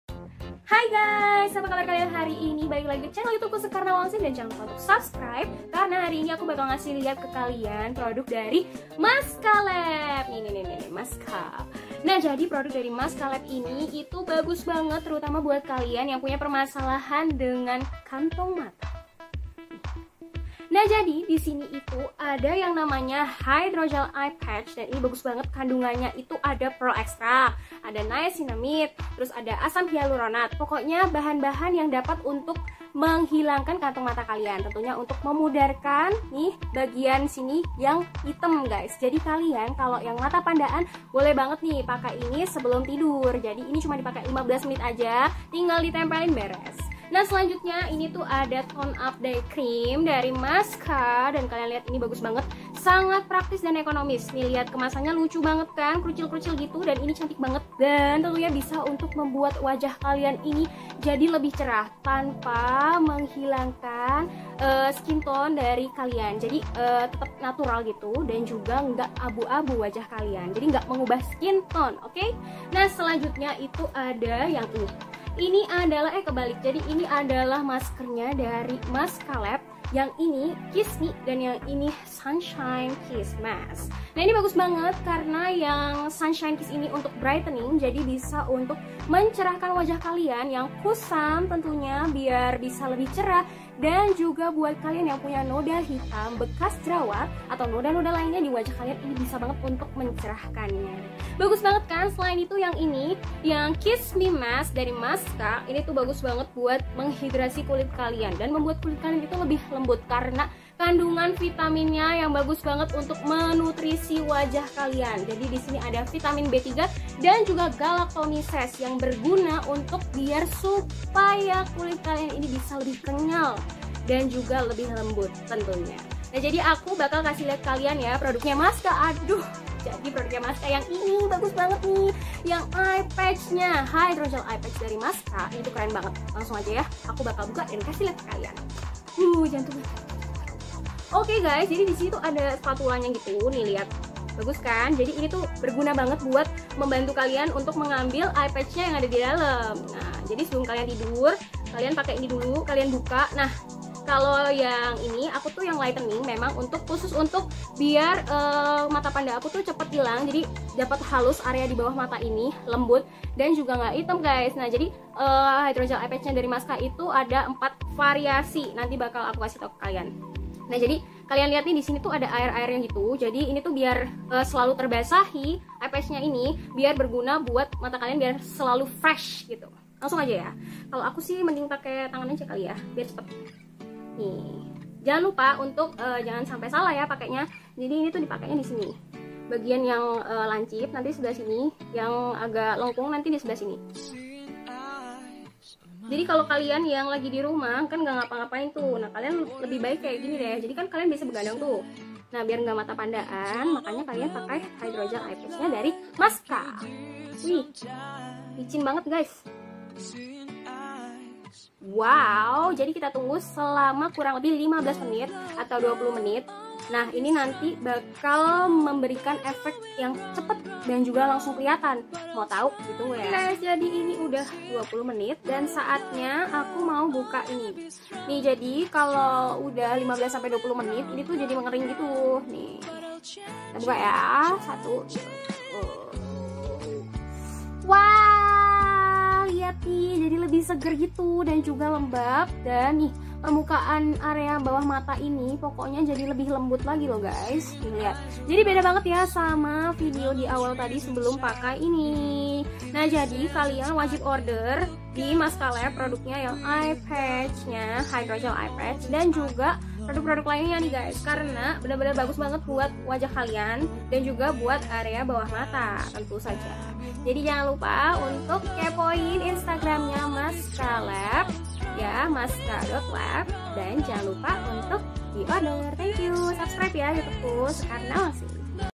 GOOD BYE MATA PANDA
Hai guys jadi aku mau ngenalin ke kalian skincare routine aku dari Maska

Jangan lupa kunjungi Instagramnya @maska.lab kunjungi juga berbagai website tentang Maska berikut ini